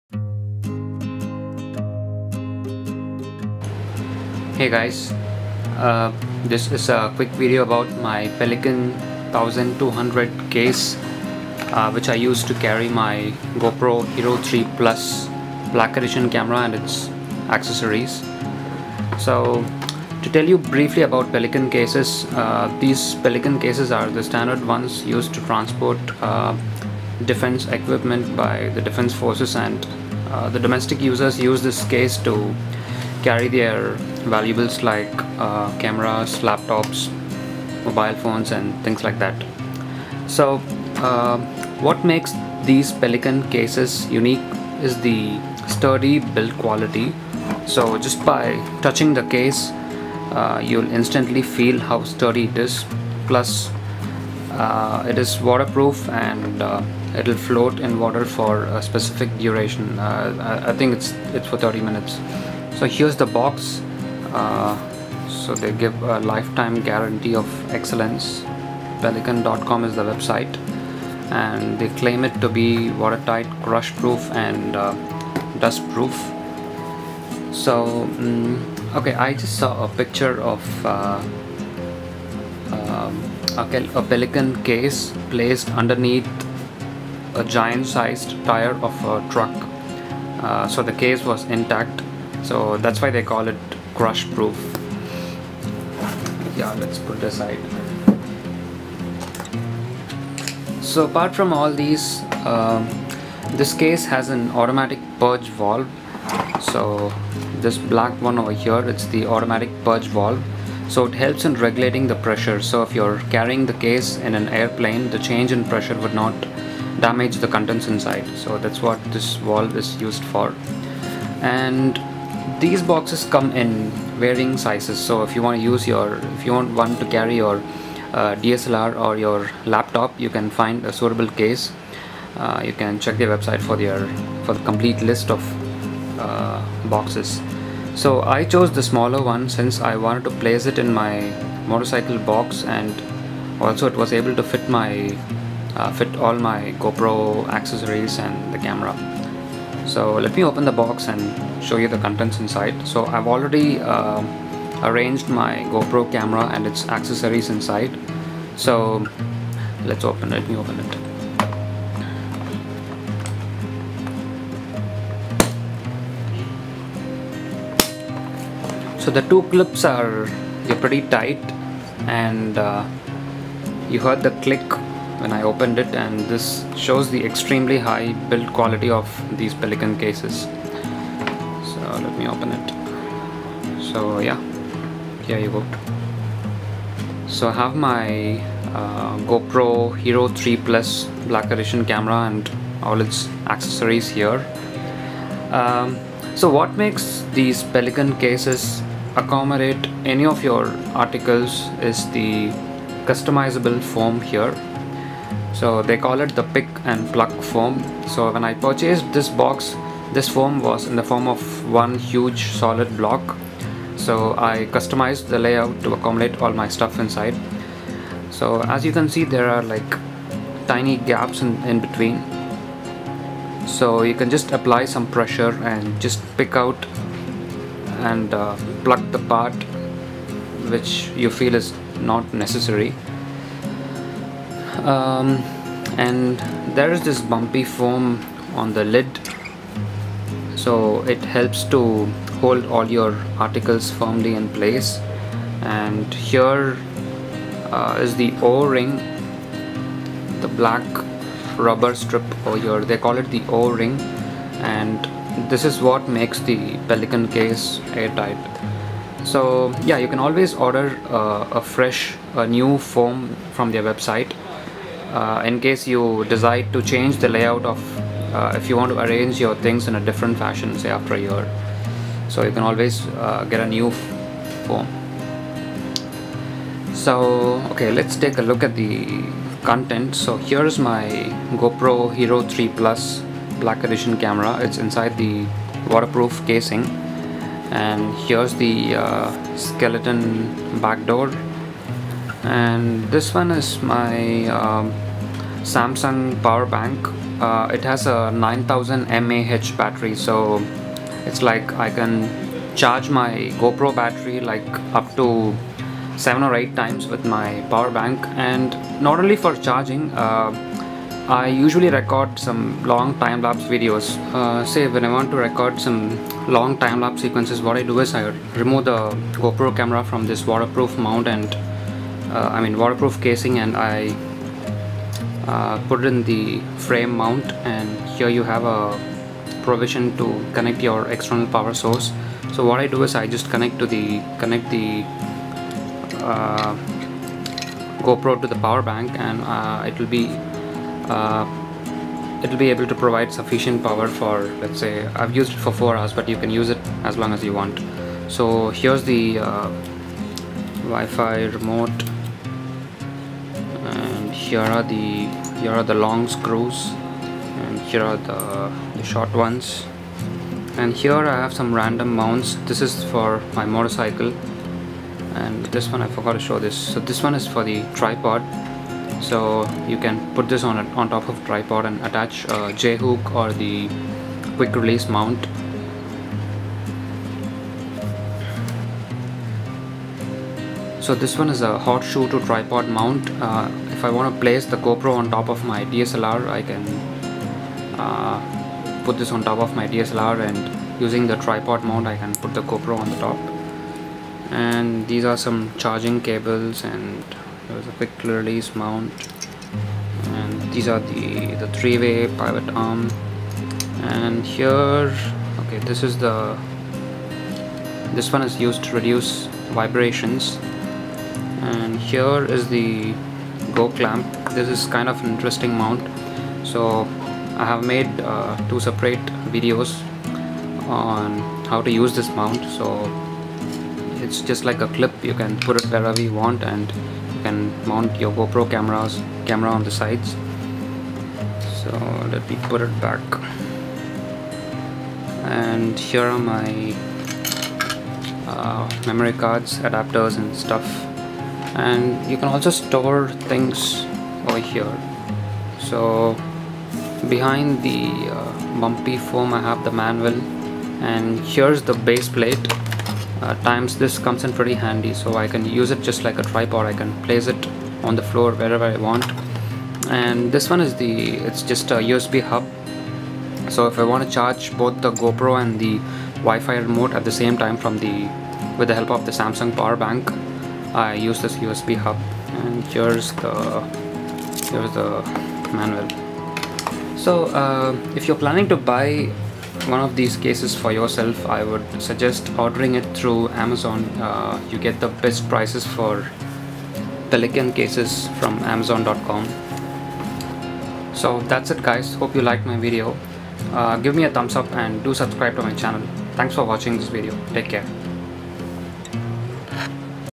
Pelican 1200 Case for GoPro Hero3+ Black edition camera
Hello YouTube. This is a quick video about the Pelican 1200 case which I use to carry my GoPro Hero 3+ Black edition camera and its mounts, accessories and other paraphernalia. The Pelican 1200 case comes with a 3 piece Pick n Pluck foam which can be customized.